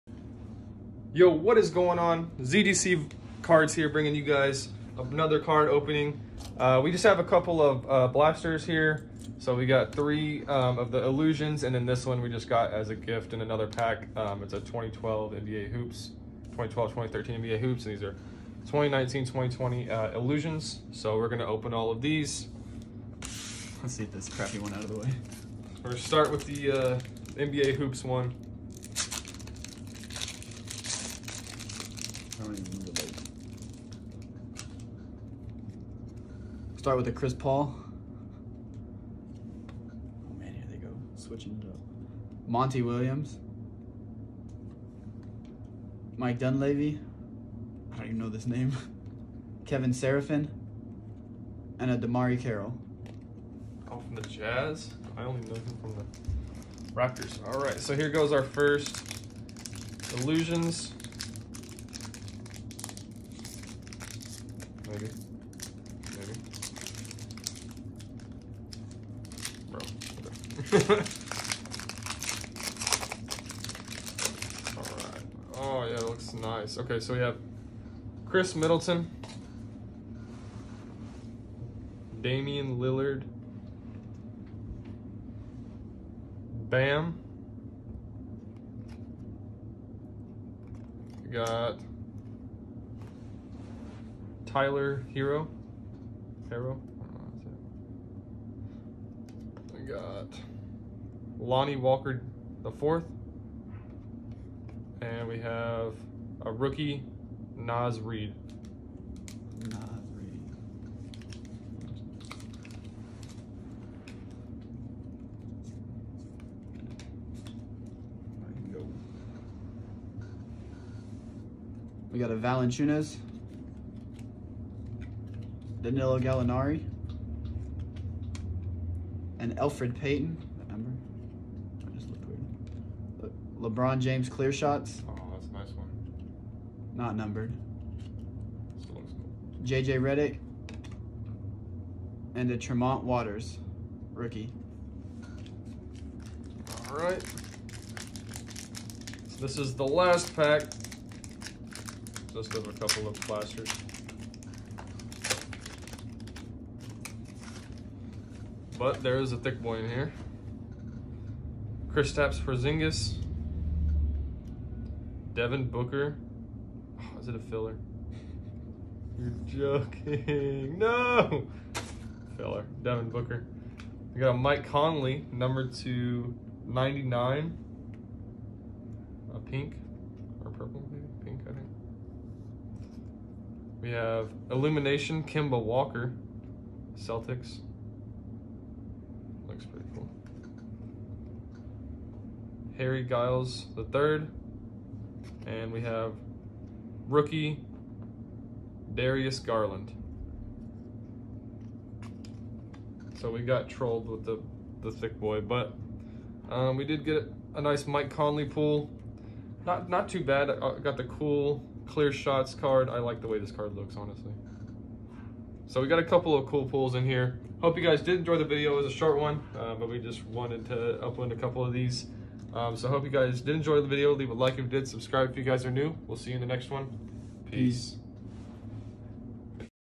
2020 Panini NBA Illusions Blasters + 2012 NBA Hoops Blaster
Items opened in this video:
- 2020 NBA Panini Illusions Blaster x 3
- 2012 NBA Hoops